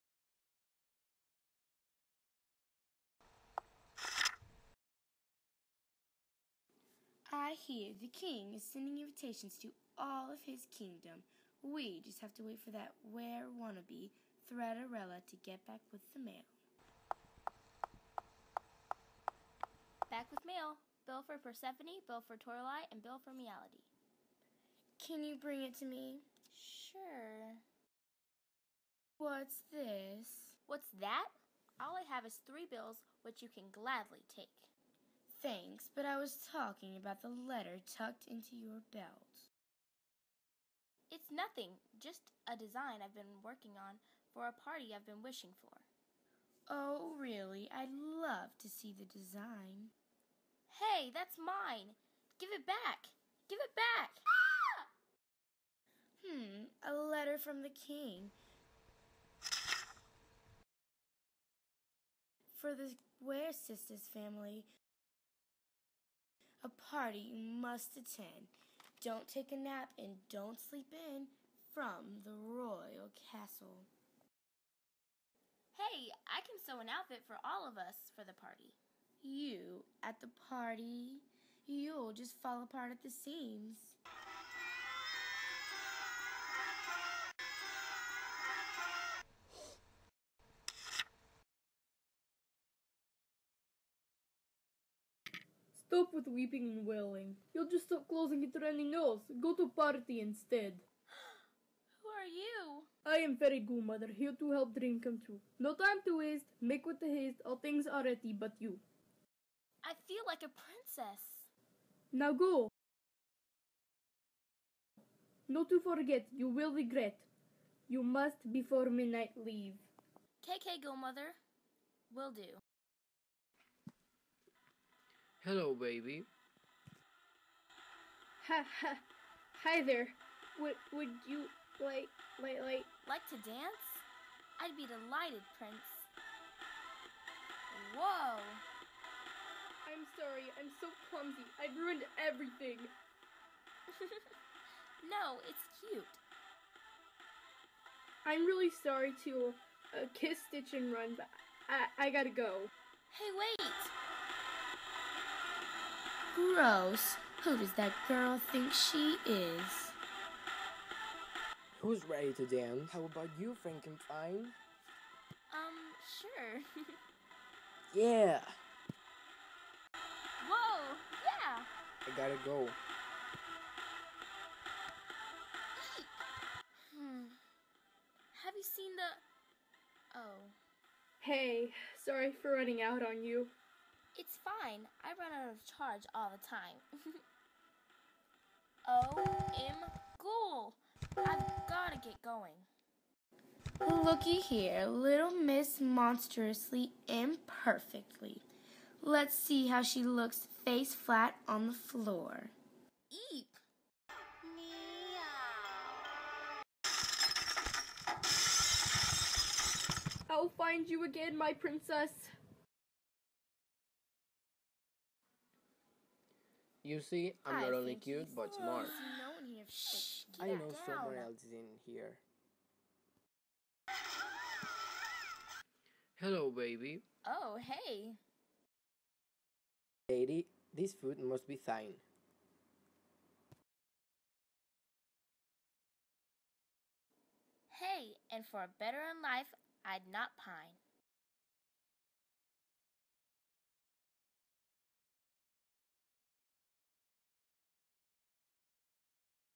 Monster High Scarily Ever After Stop Motion (FT. WookieWarrior23)
A ghoul named Threaderella (Drusila) Is stuck with her totally catty and bratty step sisters. All she wanted was to go to the ball and her caniving sisters won't let her, but with a little bolts,sparks and magic you can get anything done. Featuring Ivan (Clothesandstars) as Holt Hyde, Alexis (Nessy) as Toralei, and Chase (crummenie17) as Abbey Bominable and Jackson Jekyll!